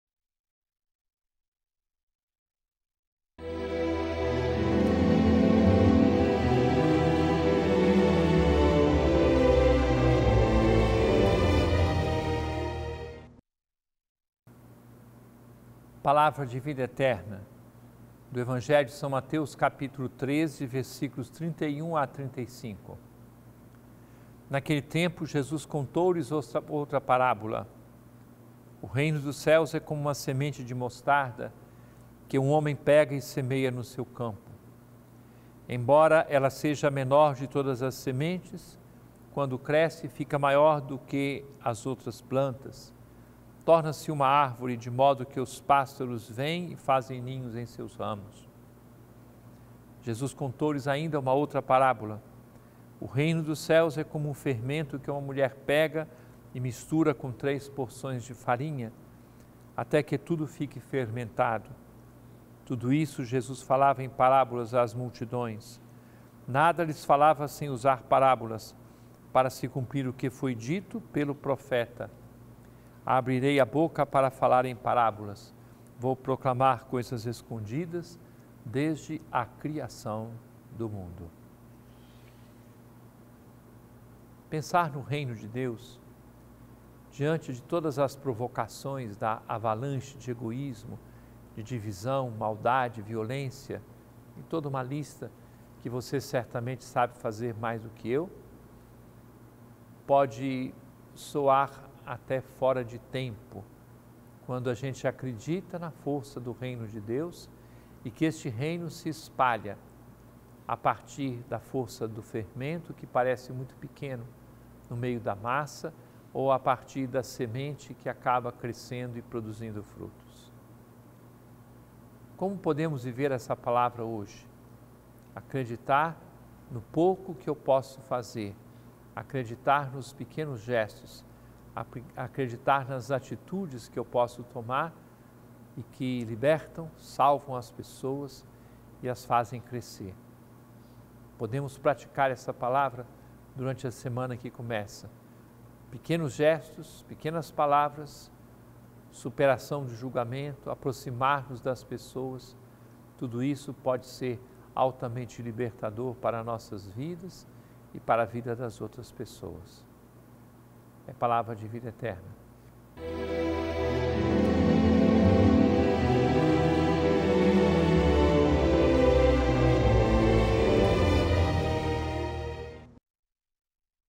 Palavra de Vida Eterna | São Mateus 13, 31-35 30/07/2018
Naquele tempo, 31Jesus contou-lhes outra parábola: “O Reino dos Céus é como uma semente de mostarda que um homem pega e semeia no seu campo. 32Embora ela seja a menor de todas as sementes, quando cresce, fica maior do que as outras plantas. E torna-se uma árvore, de modo que os pássaros vêm e fazem ninhos em seus ramos”. 33Jesus contou-lhes ainda uma outra parábola: “O Reino dos Céus é como fermento que uma mulher pega e mistura com três porções de farinha, até que tudo fique fermentado”. 34Tudo isso Jesus falava em parábolas às multidões. Nada lhes falava sem usar parábolas, 35para se cumprir o que foi dito pelo profeta: ‘Abrirei a boca para falar em parábolas; vou proclamar coisas escondidas desde a criação do mundo’.